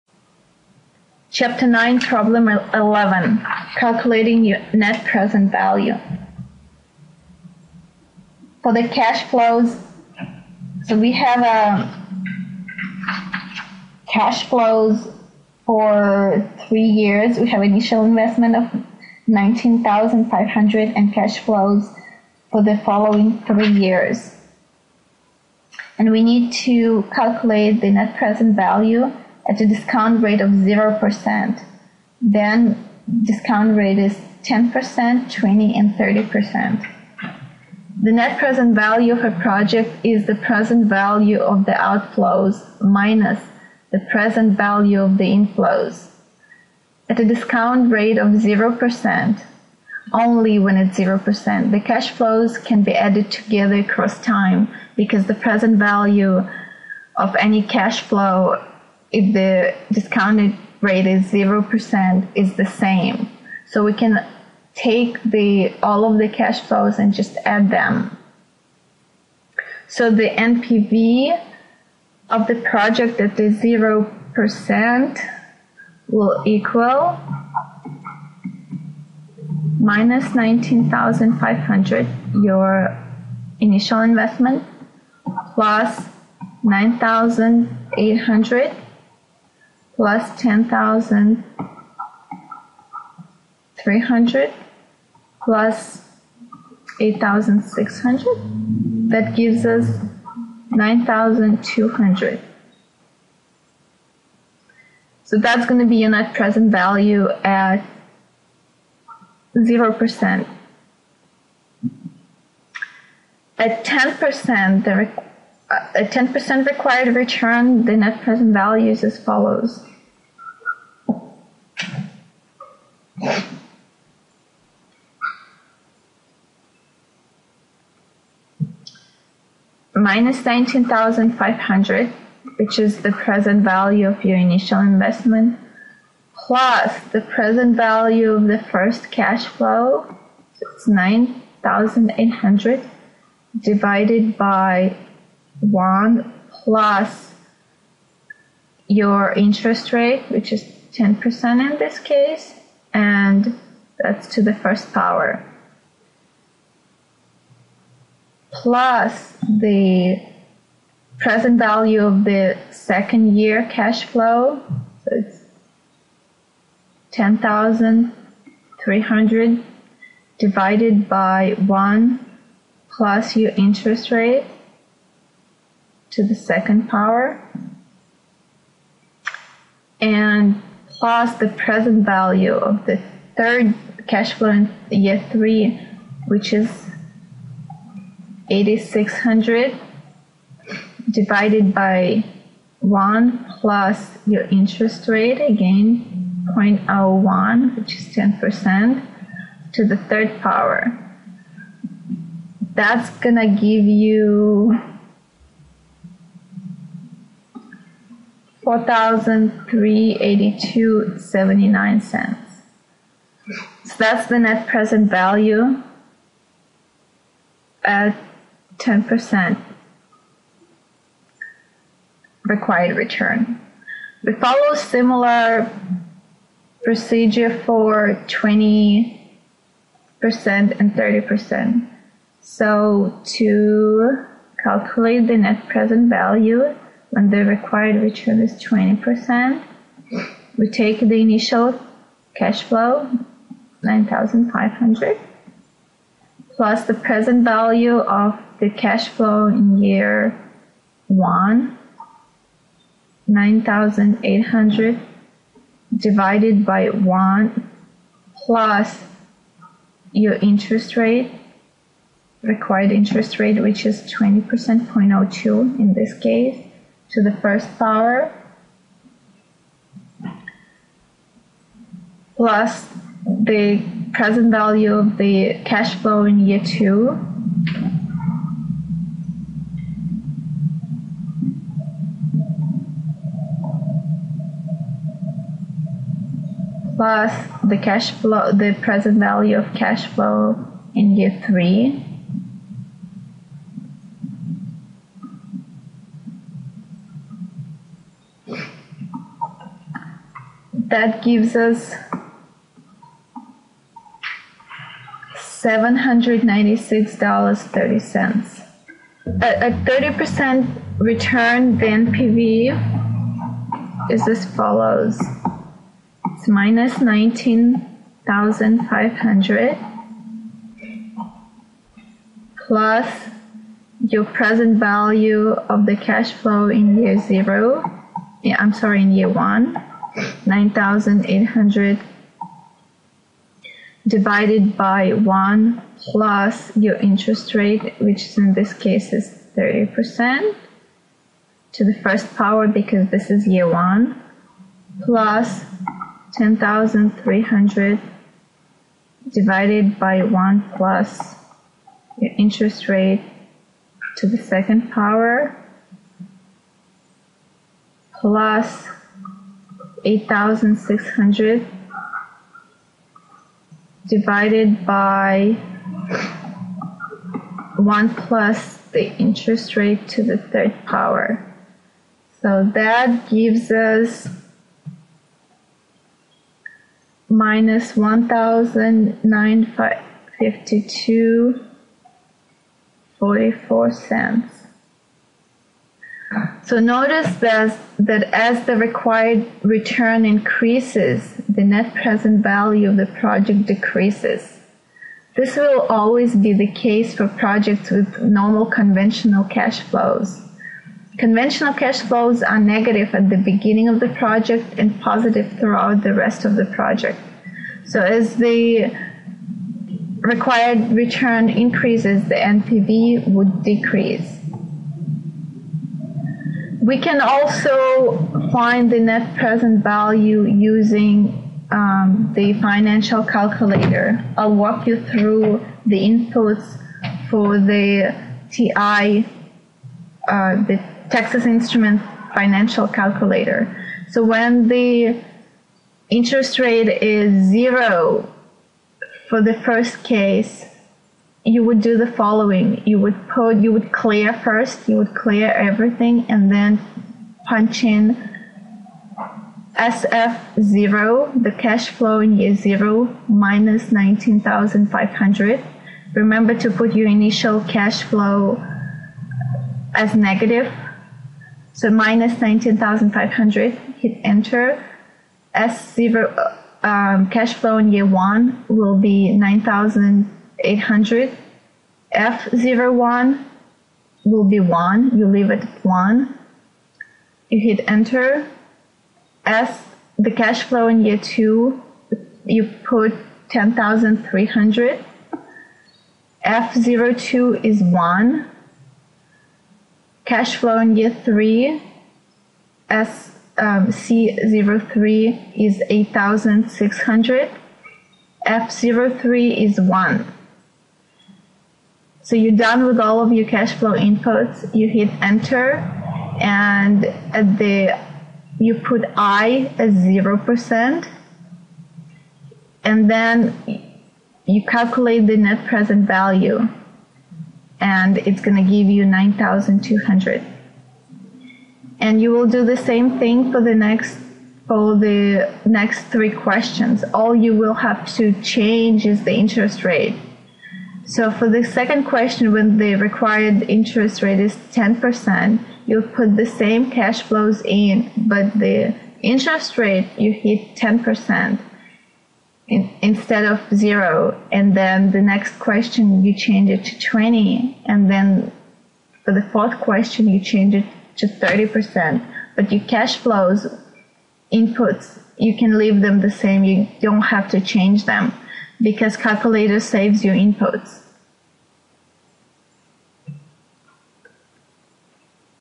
Net Present Value at Various Discount Rates
Fundamentals of Corporate Finance by Ross, Westerfield, and Jordan.
Chapter 9 Problem 11: Calcultaing NPV at a discount rate of 0%, 10%, 20%, and 30%.